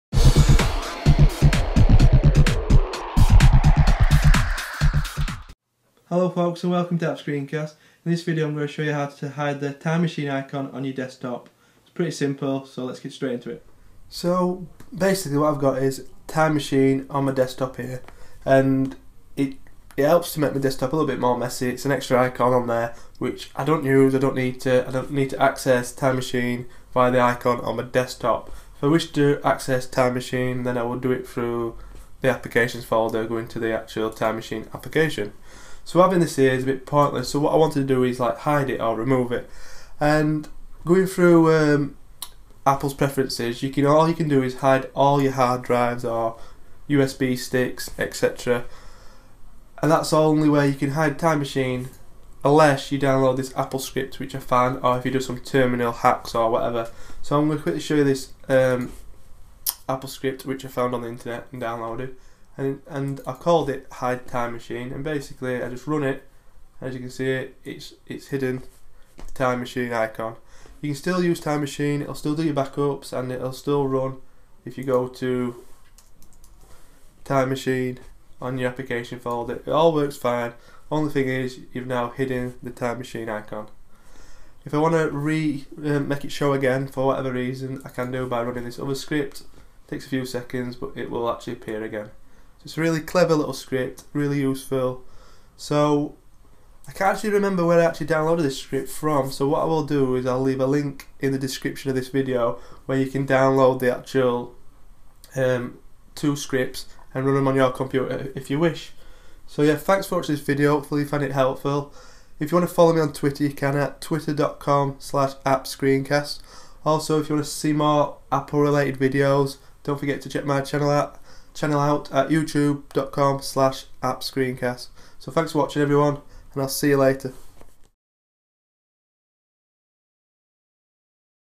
Hide Time Machine icon on the desktop (mac)
How to hide only the time machine icon on your desktop.